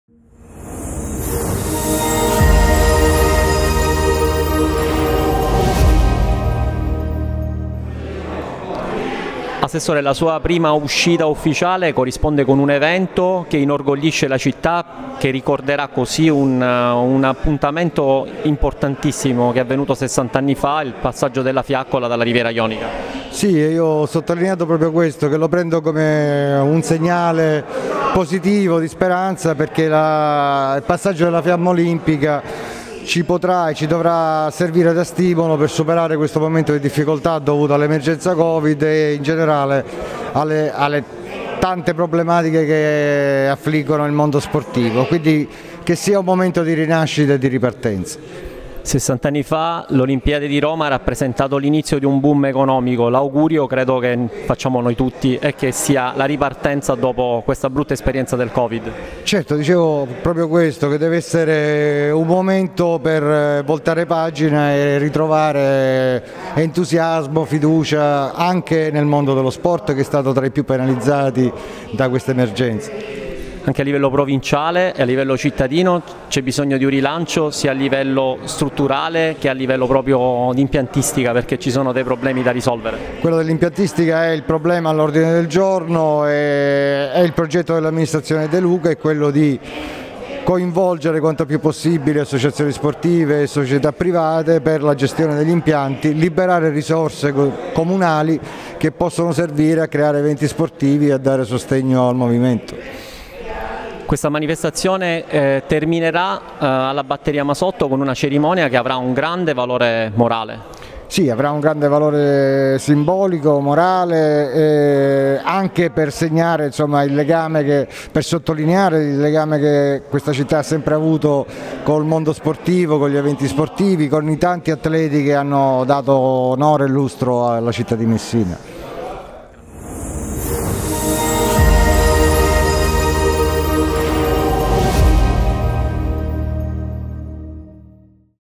Intervista all'assessore allo sport Francesco Gallo
60° dal passaggio della Fiaccola Olimpica